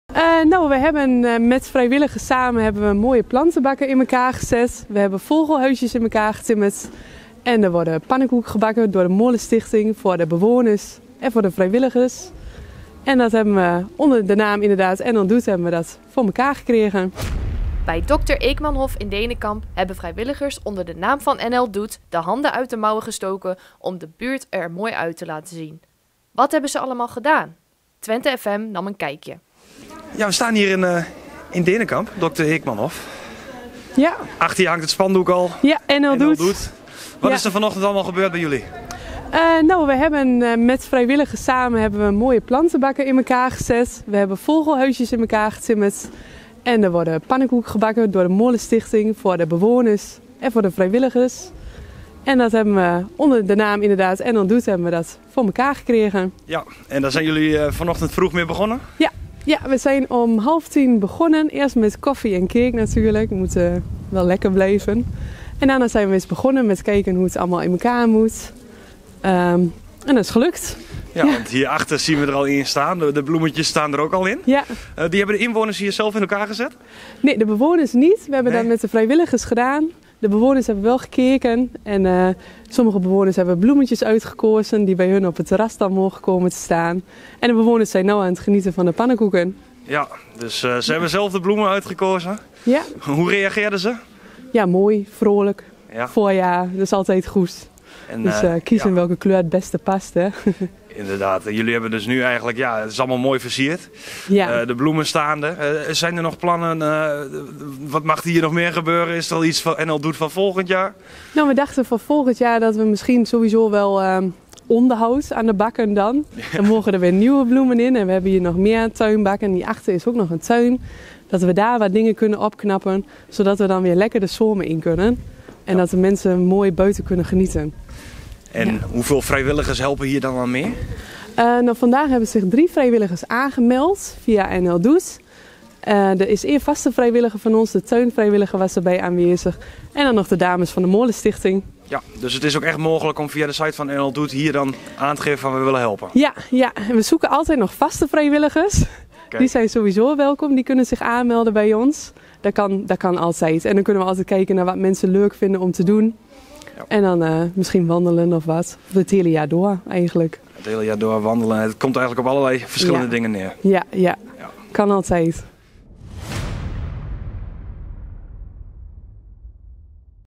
NL Doet op Dr. Eekmanhof in Denekamp - Twente TV
Ook op de Dr. Eekmanhof in Denekamp werd er vorige week druk gewerkt tijdens NL Doet. Er werden nieuwe bloembakken gemaakt met daarin een aantal mooie bloemen en na de tijd werden er pannekoeken gemaakt voor de bewoners.